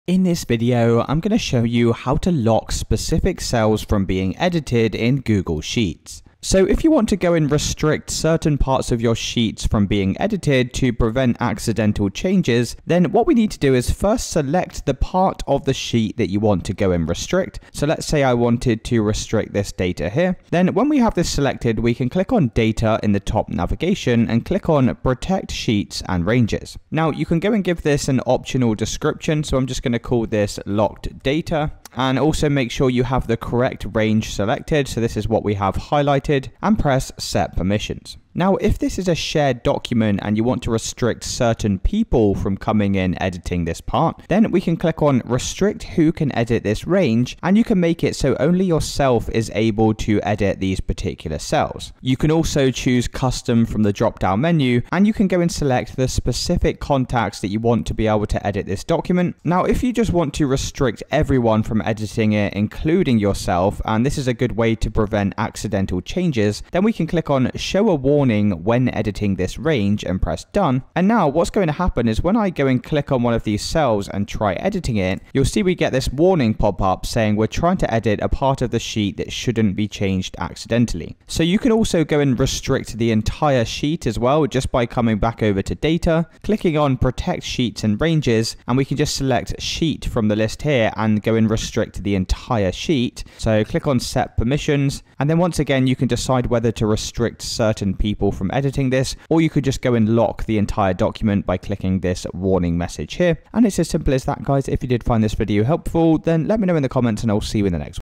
How To Lock & Restrict Cells From Editing in Google Sheets
In this video, learn how to lock and restrict cells from editing in Google Sheets to protect important data. Whether you're managing a shared spreadsheet or preventing accidental changes, this guide will show you how to lock specific cells, set editing permissions, and keep your data secure.